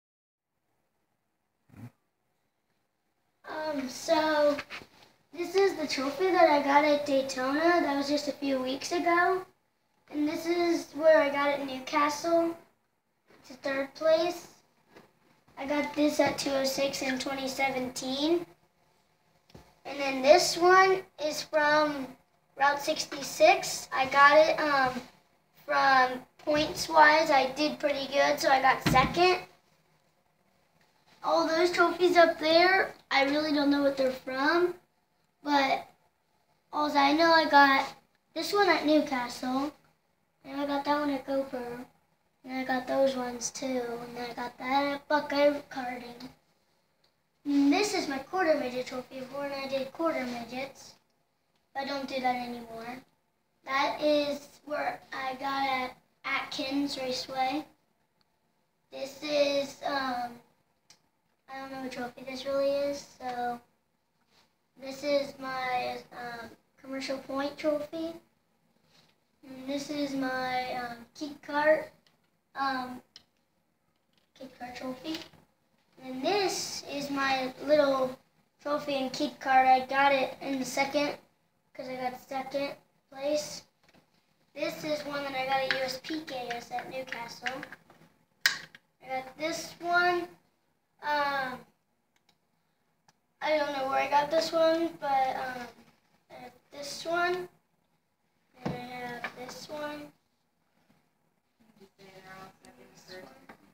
Hilliard's Aryton Grim
Aryton Grim, age 9, shows some of the trophies he has won in go-kart racing. [AlanFroman/ThisWeek]